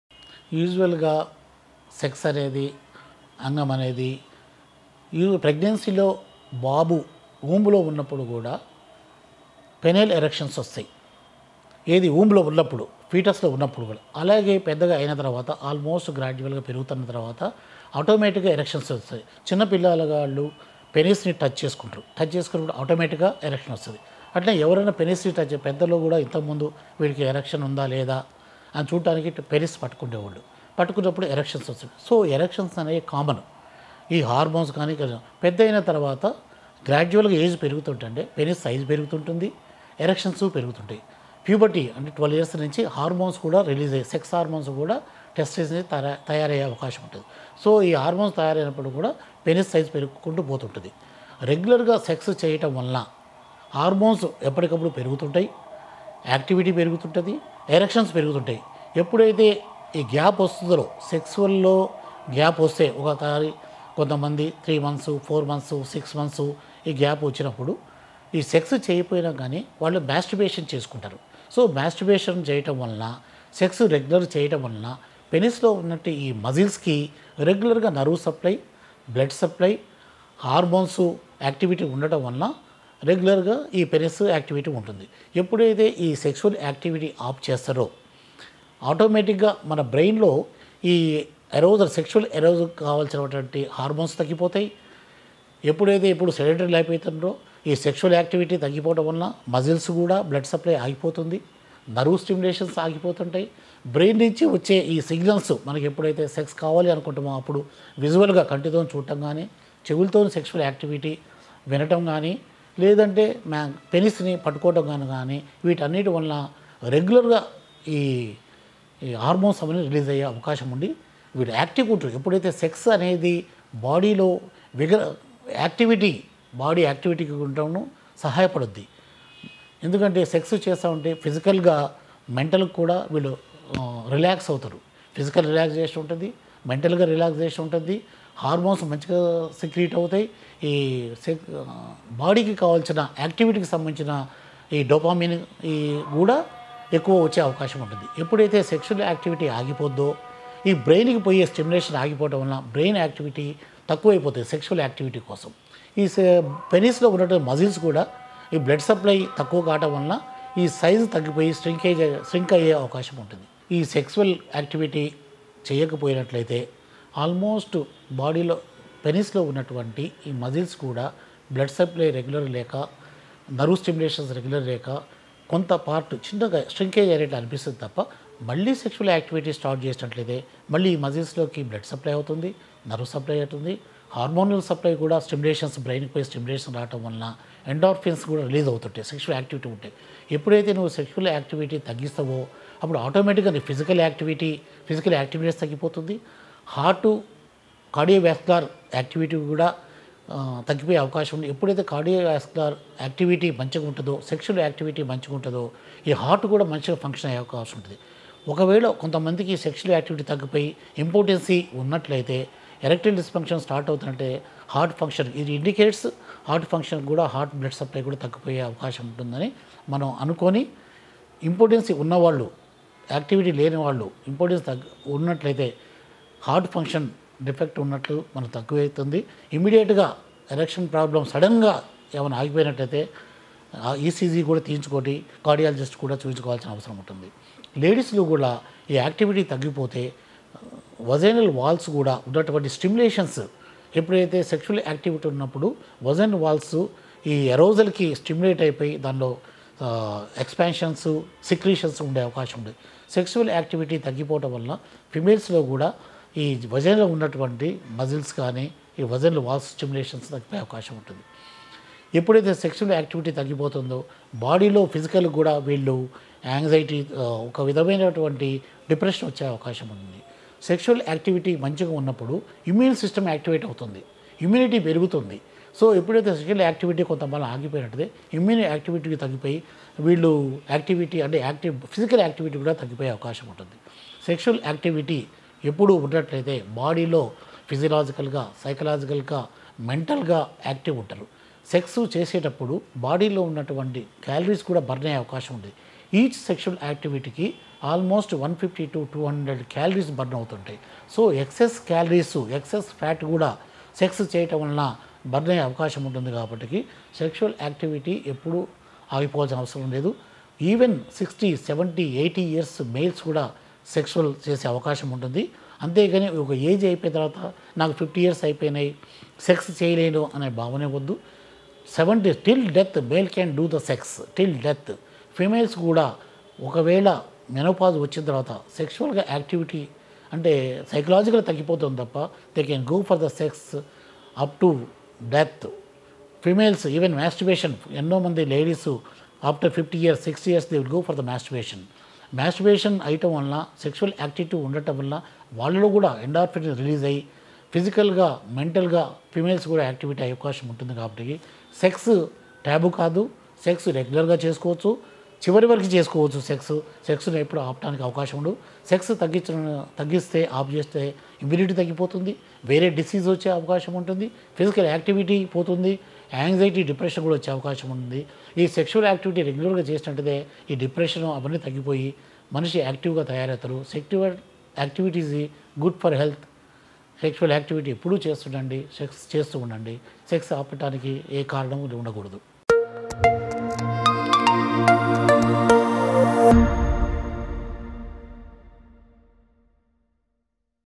Natural Ways to Treat Erectile Dysfunction | How to have Stronger Erections? | Men's Health Telugu
Natural Ways to Treat Erectile Dysfunction | How to have Stronger Erections?  | Men's Health Telugu | Dr K Bendadi Kumar | TX TV